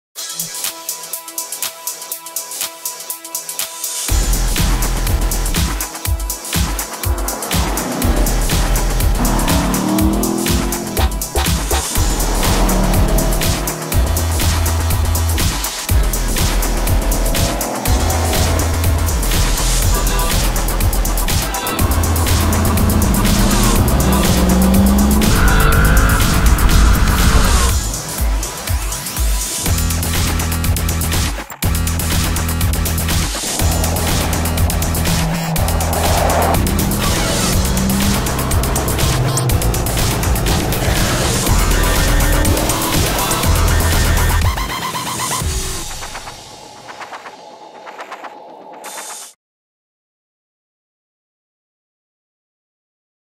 AudiBlog nl Presents: The Audi RS 5 Cabriolet Trailer
Trailer of the Audi RS 5 Cabriolet driving on mountain roads.
Original by Audi AG/quattro GmbH